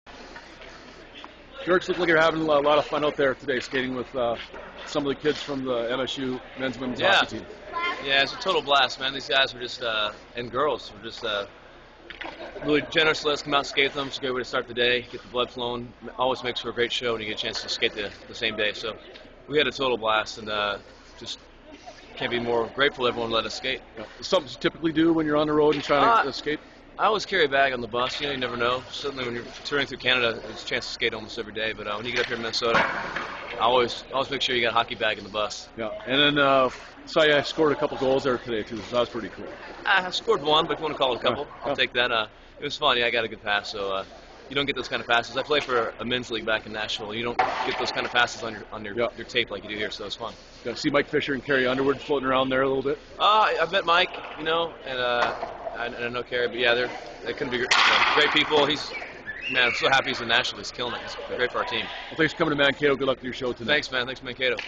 Dierks Bentley plays hockey in Mankato
Country recording artist Dierks Bentley, scheduled to perform at Verizon Wireless Center in Mankato April 21, 2011, scrimmaged with members of the Minnesota State men's and women's hockey team and several local amateur players in the afternoon.